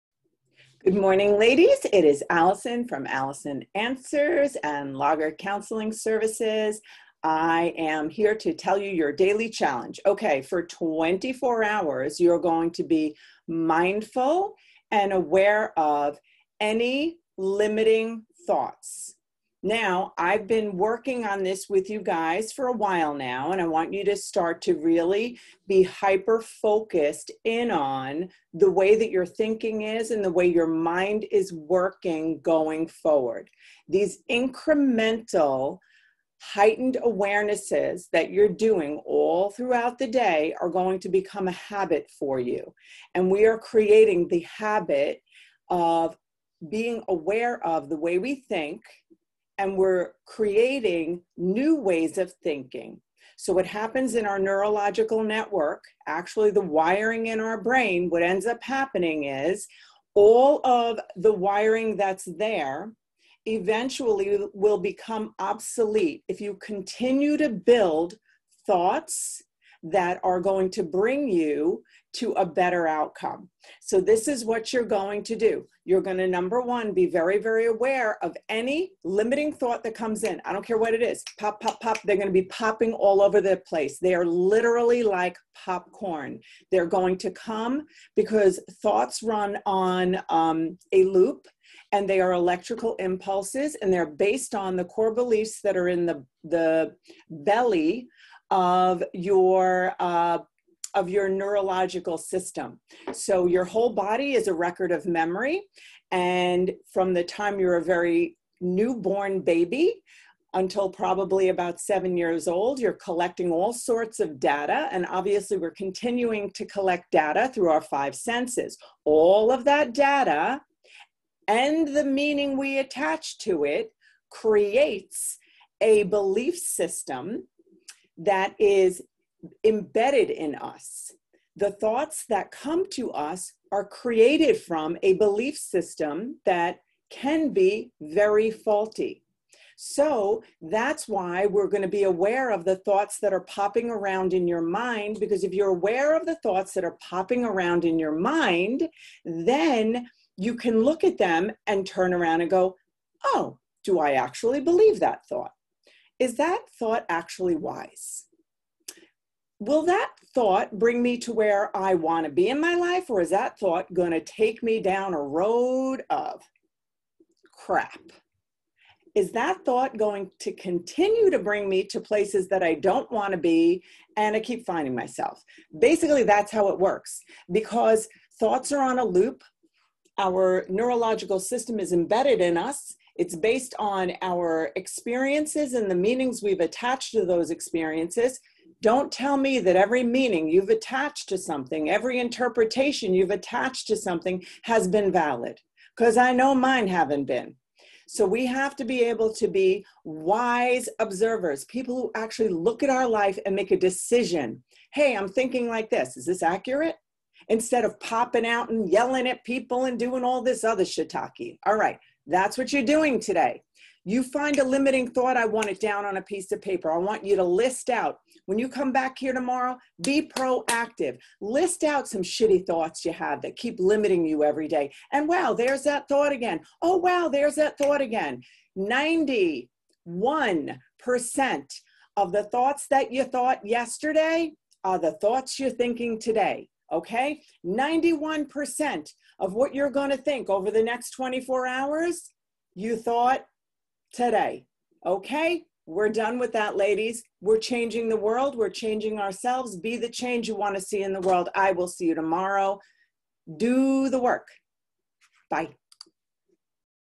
Quick Ways to Change Your Negative Thinking
Todays daily dose is about being mindful and aware of any limiting thoughts you have